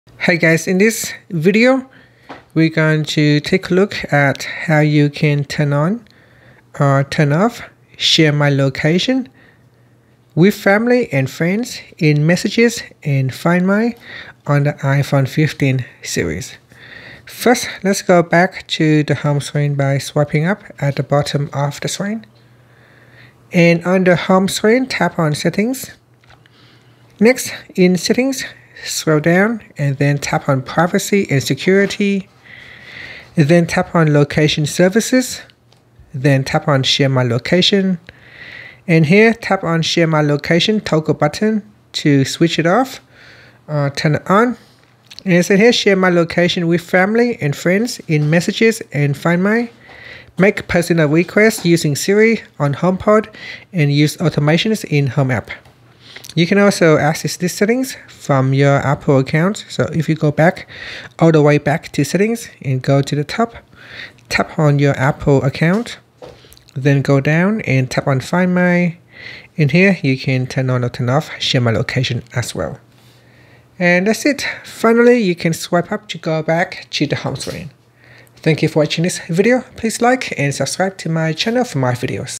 iPhone 15/15 Pro Max: How to Turn On/Off Share My Location With Family and Friends
Welcome to our iPhone 15 series tutorial! In this video, we'll show you the ins and outs of sharing your location with family and friends on the latest iOS 17. Learn how to seamlessly turn on and off location sharing on your iPhone 15, iPhone 15 Pro, iPhone 15 Pro Max, and iPhone 15 Plus. Whether you're coordinating meetups or ensuring your loved ones can always find you, these steps are essential for getting the most out of your device.